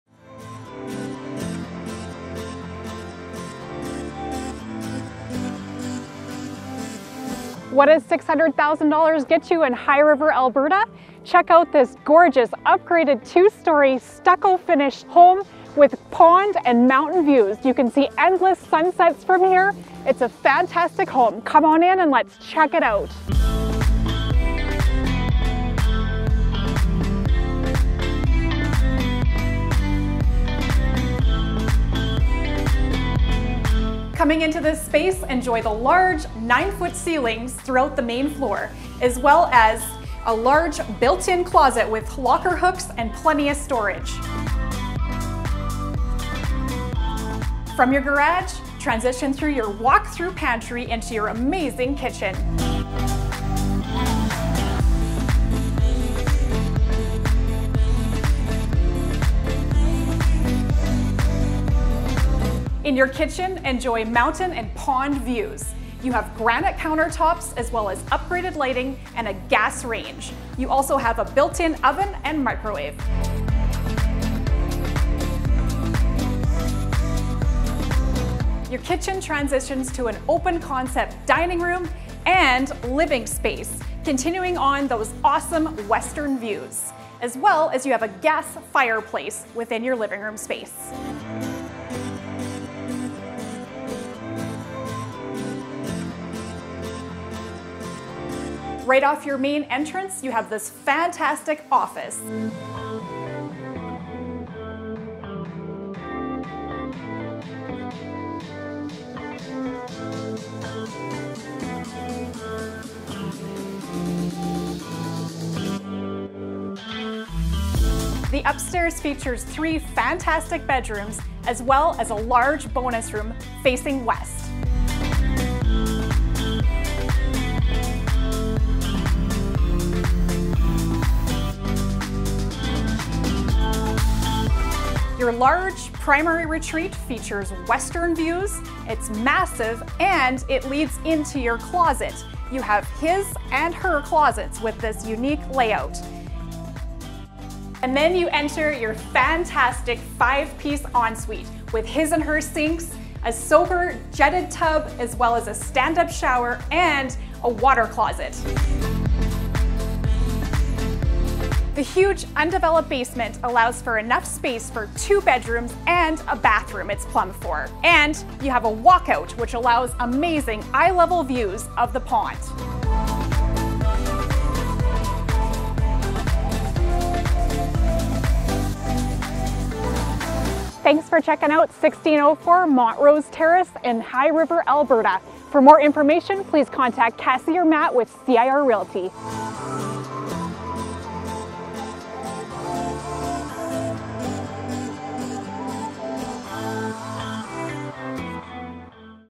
High River Real Estate Property Video Tour Production - 1604 Montrose Terrace SE
1604 Montrose Terrace SE, High River
This property is listed by
Cassie & Matt Real Estate
CIR Realty